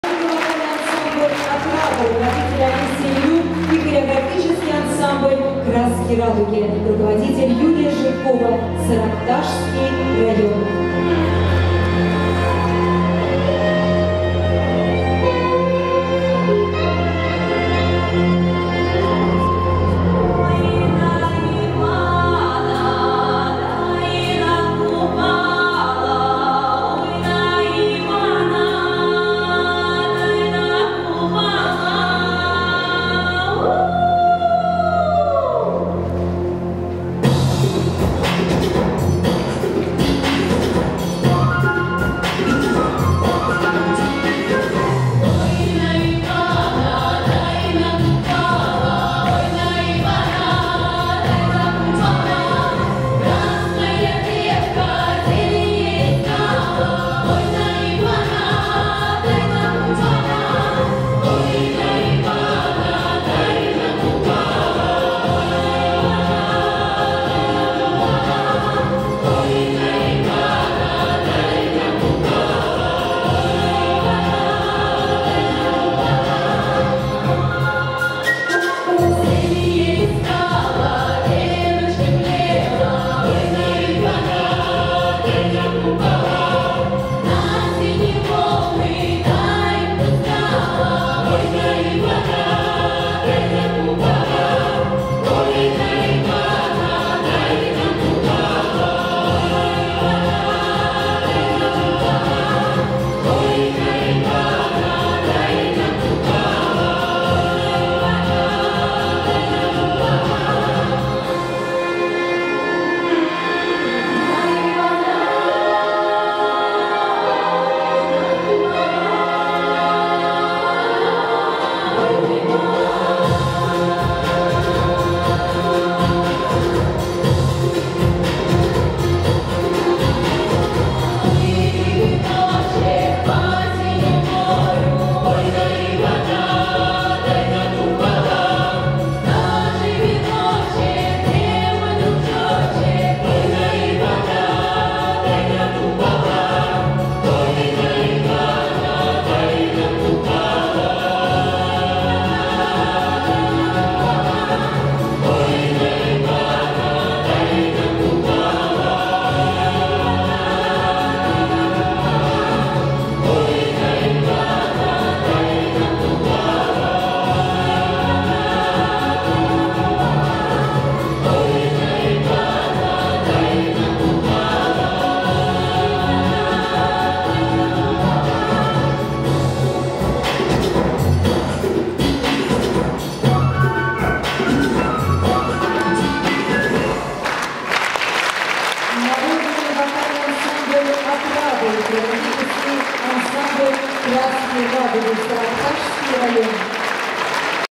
Народный вокальный ансамбль "Отрада" и хореографический ансамбль "Краски радуги"
Народный вокальный ансамбль "Отрада"
Руководитель Олессия Лелюк и
Хореографический ансамбль "Краски радуги"
Руководитель Юлия Жидкова
Саракташского РДЦ
Гала концерт "Обильный край, благословенный!" 2019